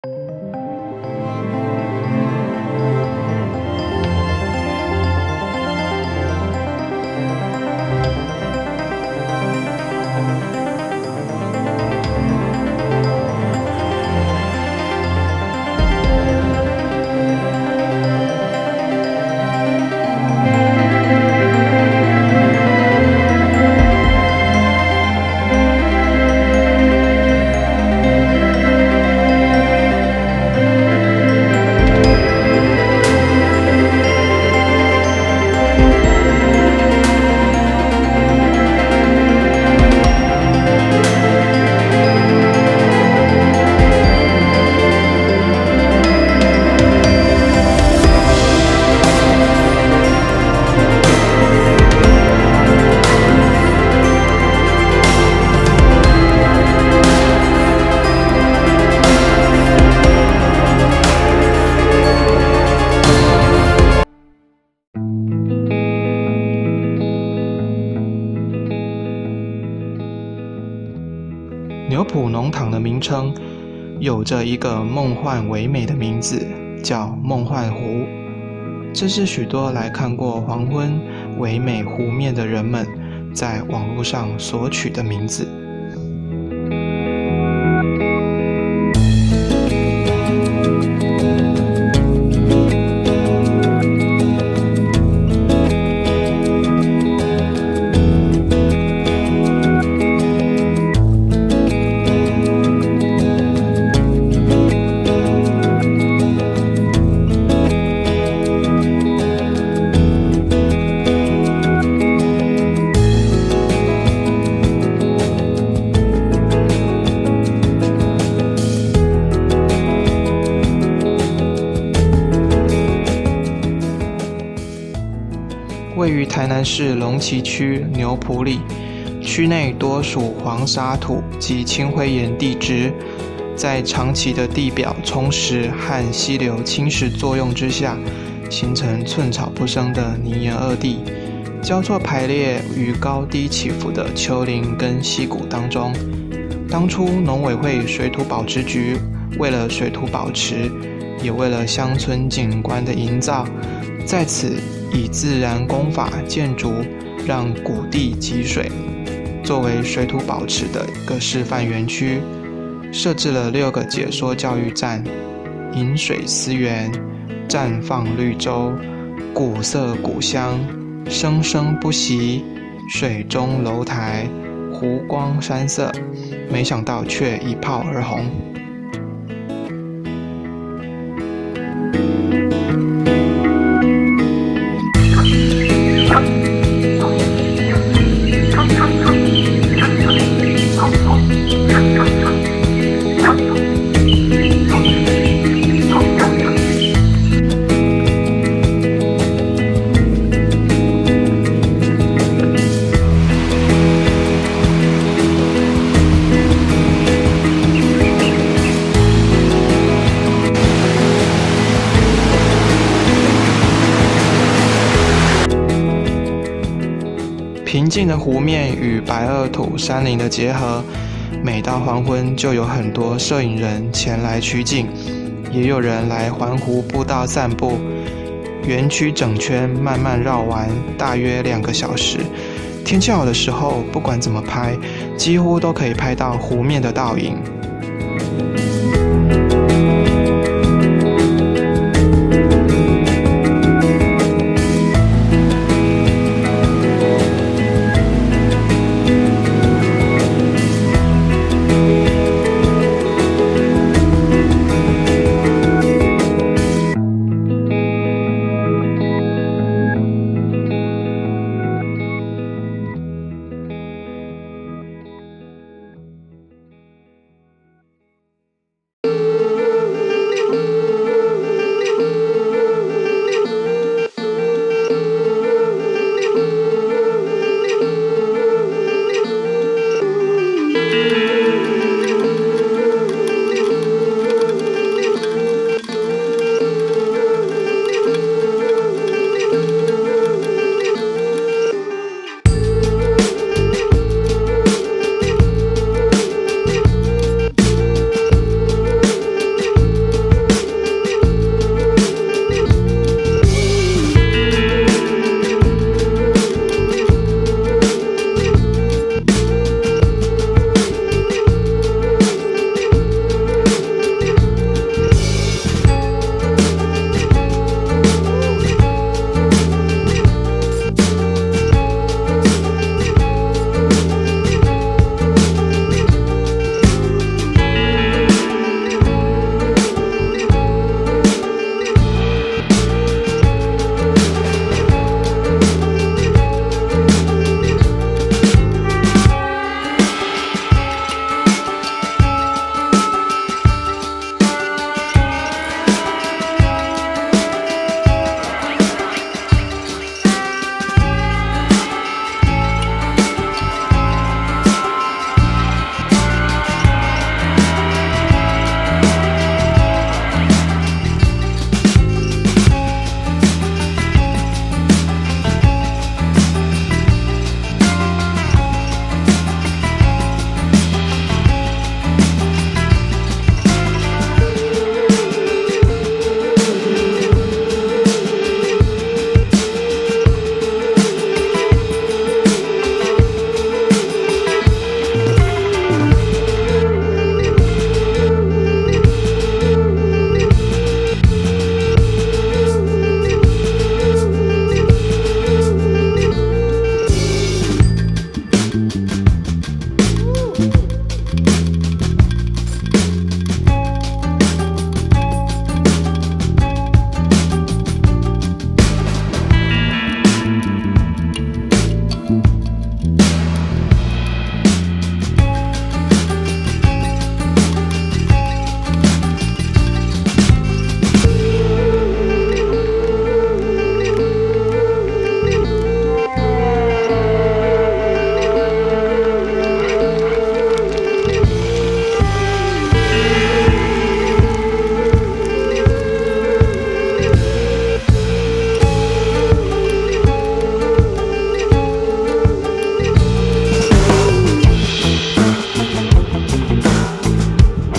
【惡地中的夢幻湖】台南龍崎牛埔農塘（沙子在旅行帶您一起環湖欣賞惡地美景、水土保持與生態共存！）疫情前拍攝，歡迎按讚與分享！
牛埔農塘 的名稱，有著一個夢幻唯美的名字叫「夢幻湖」，這是許多來看過黃昏唯美湖面的人們在網路上所取的名字。位於台南市龍崎區牛埔里，區內多屬黃砂土及青灰岩地質，在長期的地表沖蝕和溪流侵蝕作用之下，形成寸草不生的泥岩惡地，交錯排列於高低起伏的丘陵與溪谷中，當初農委會水土保持局為了水土保持、也為了鄉村景觀的營造，在此以自然工法建築讓谷地集水，作為水土保持的示範區，設置了六個解說教育站：「飲水思源」、「綻放綠洲」、「古色古香」、「生生不息」、「水中樓台」、「湖光山色」沒想到卻一炮而紅，平靜的湖面與白堊土山陵的結合，每到黃昏就有很多攝影人前來取景，也有人來環湖步道散步，園區整圈慢慢繞完，大約2個小時。天氣好的時候，不管怎麼拍，幾乎都可以拍到湖面的倒影，春天花開季節到訪的話，在園區步道旁隨意都可以捕捉花叢與平靜水面的倒影畫面，如果體力許可，建議走到步道頂端，是眺望整個區域的最佳地點。